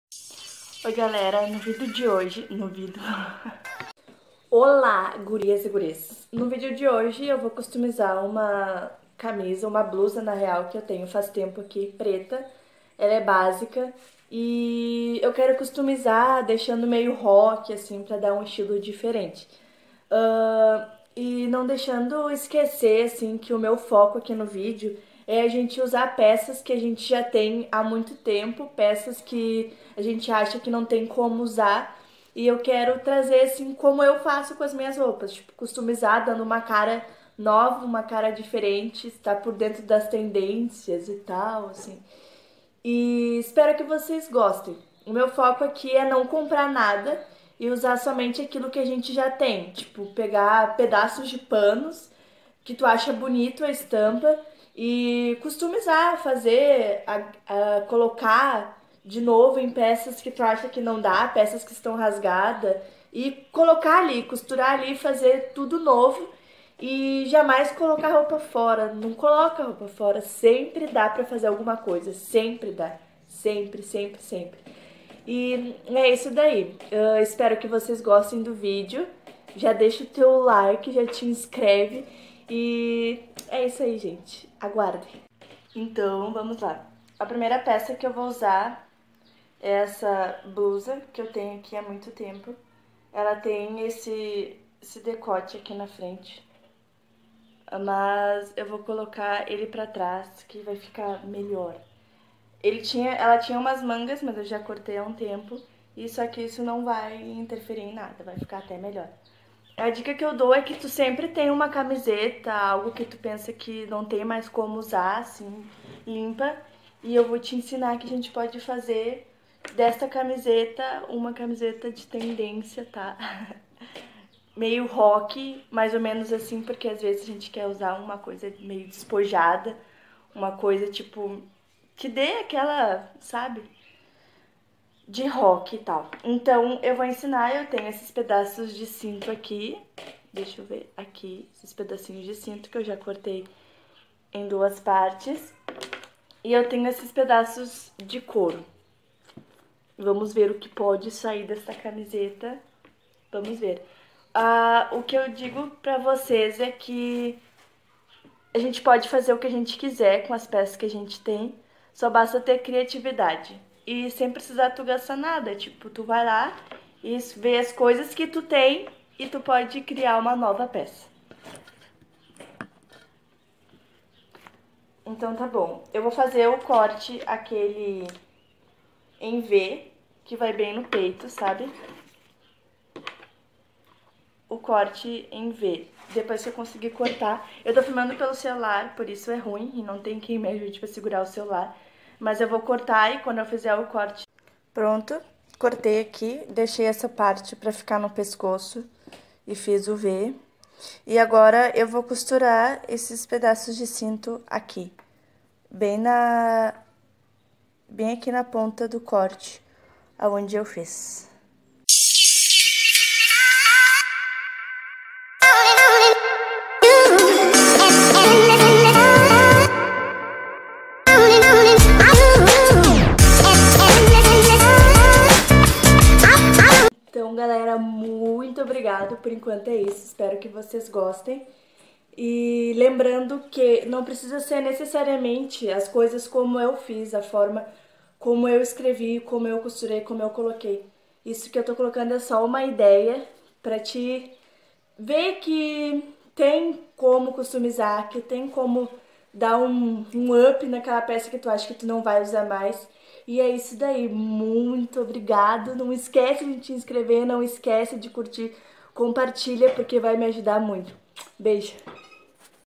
Customizado aquela blusa velhinha.
Desde já peço perdão pelas bordas, é que eu gravei com o celular em pé e esqueci que não podia.
Sei que com o tempo vou melhorando, muito obrigada a todos de coração. ⚘